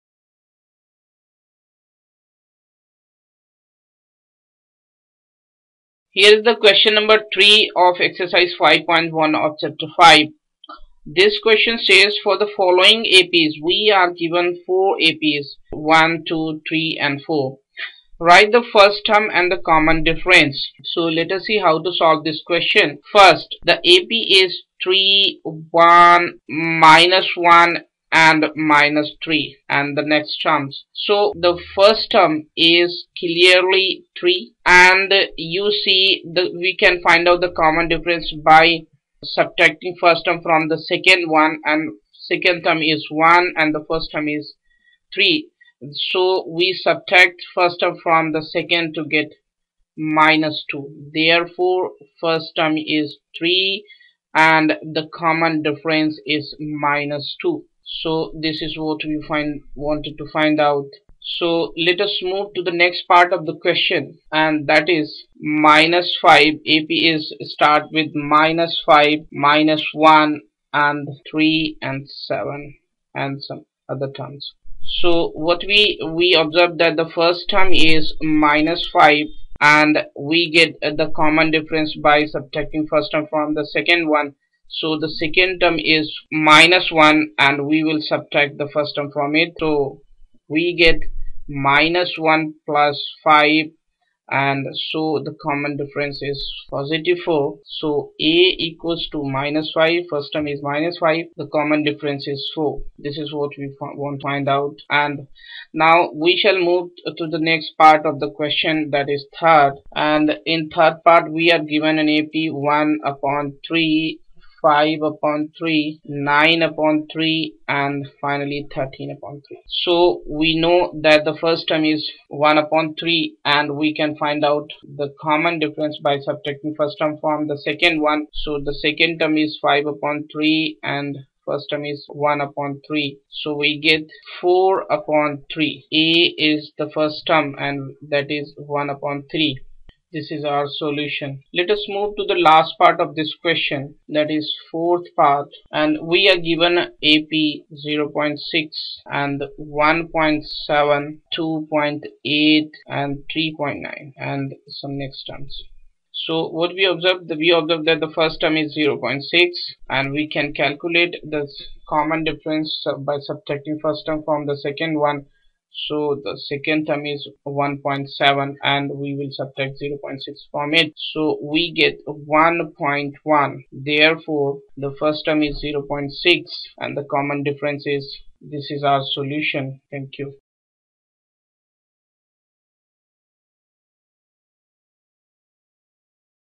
Ex 5.1 : Q.3 : For the following APs, write the first term ... Ch 5 | Class Xth Math for CBSE
In this video, we have calculated first term and common difference of given APs.

All videos from Chapter 5